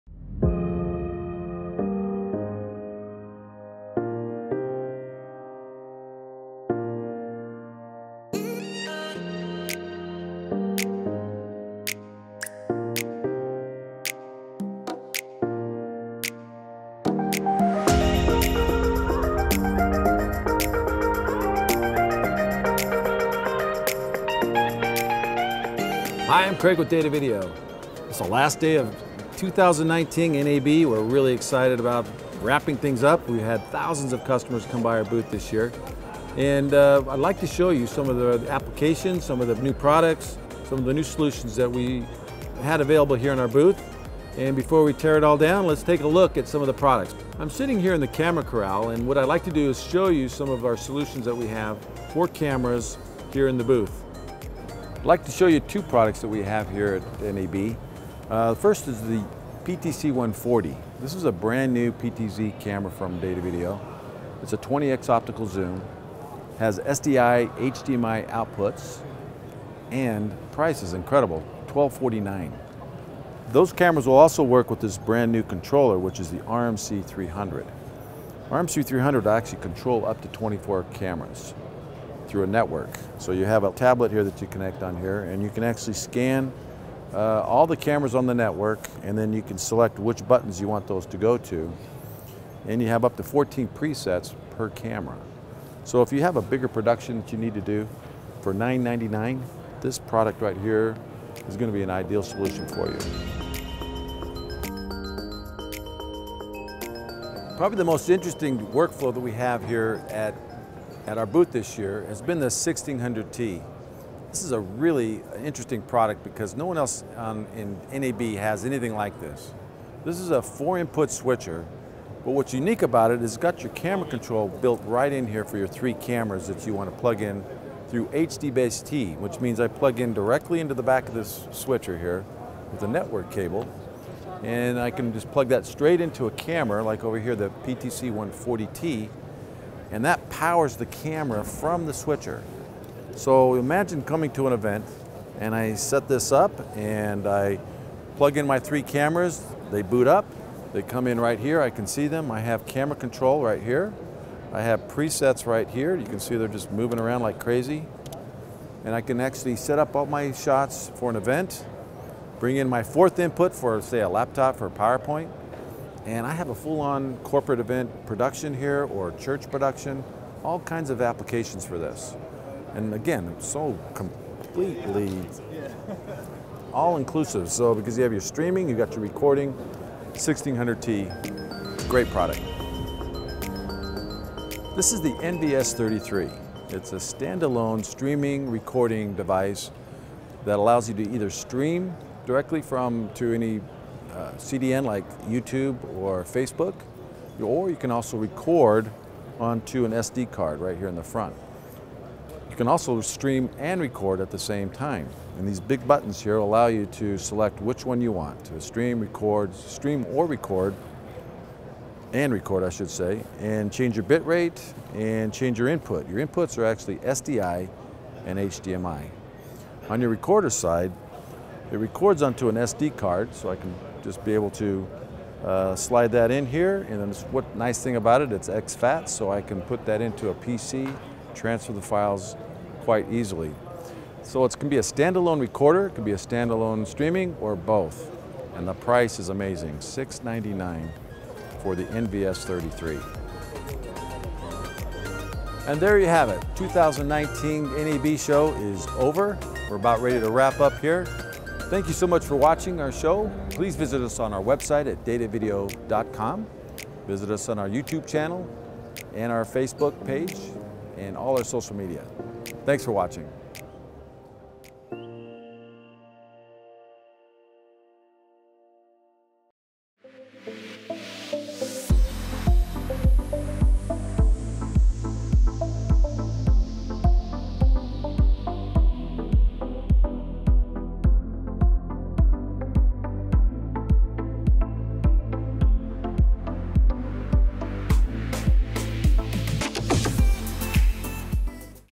New Datavideo Products at NAB 2019 | Mobile Production, Cameras, Streaming
Craig Moffat introduces Datavideo's new products at the 2019 NAB Show in Las Vegas: The HS-1600T Mobilecast HDBaseT Switcher with built-in streaming and recording, PTC-140 and PTC-140T Pan-tilt-Zoom Cameras, RMC-300C Camera Controller, and NVS-33 Streaming Encoder / Recorder.